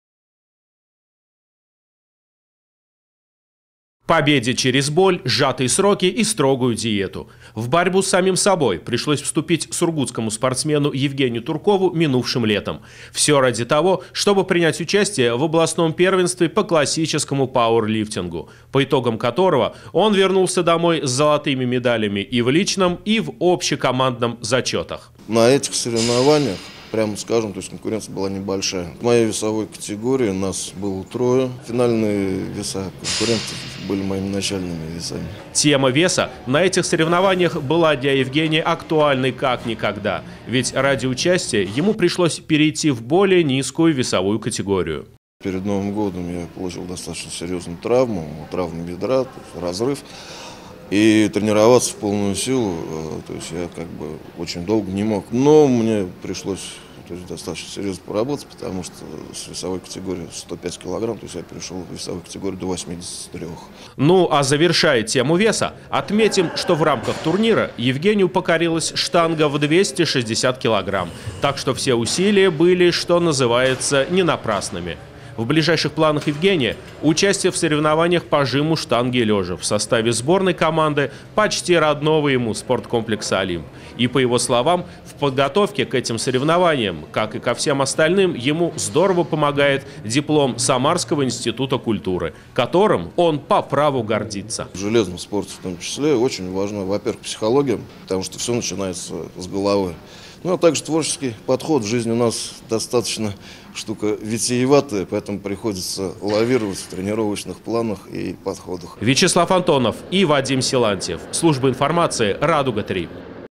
На своих плечах
Сургутский спортсмен-ветеран Евгений Турков стал победителем личного и общекомандного зачётов Первенства Самарской области по пауэрлифтингу